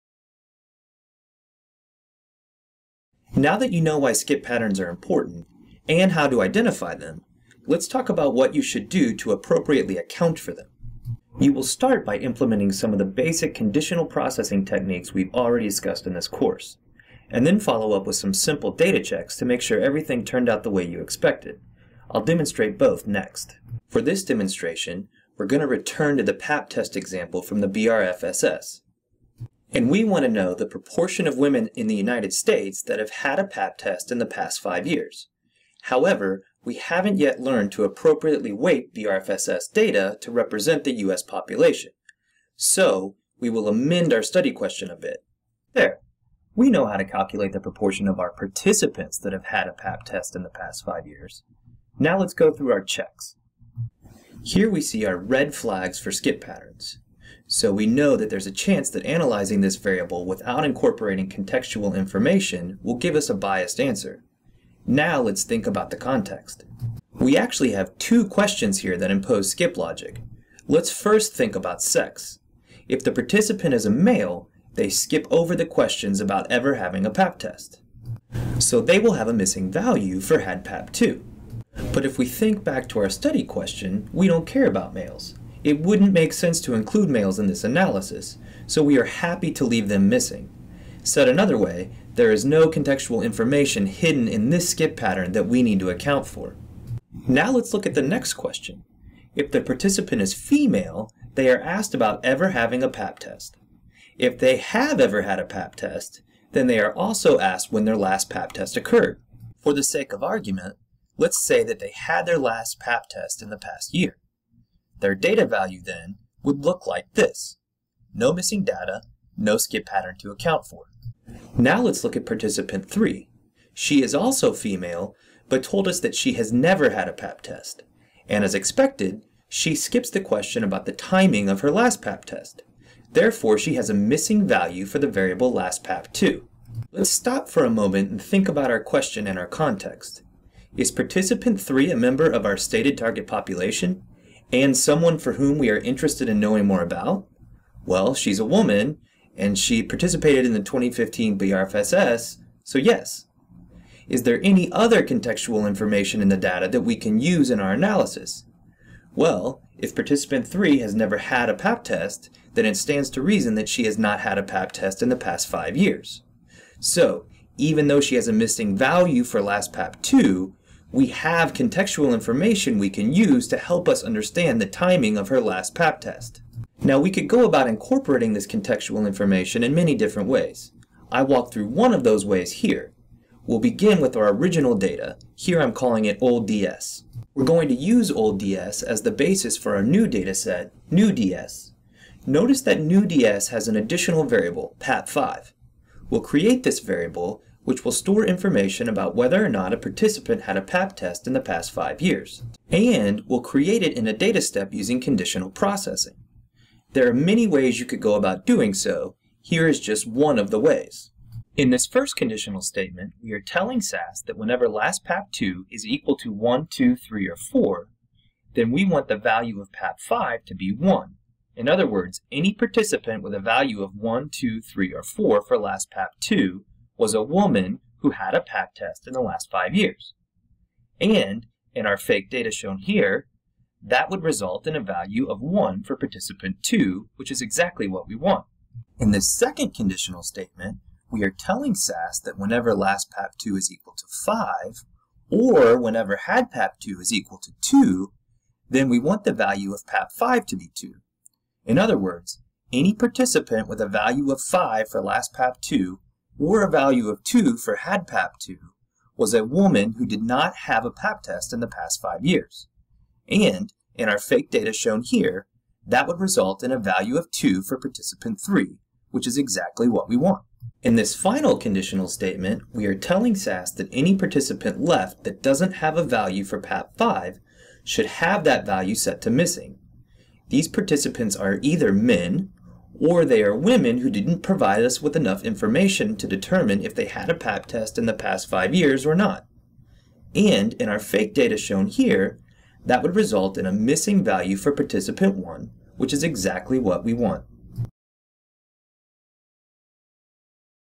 Managing Skip Patterns
In this video, we discuss some basic data management techniques for dealing with skip patterns in your data.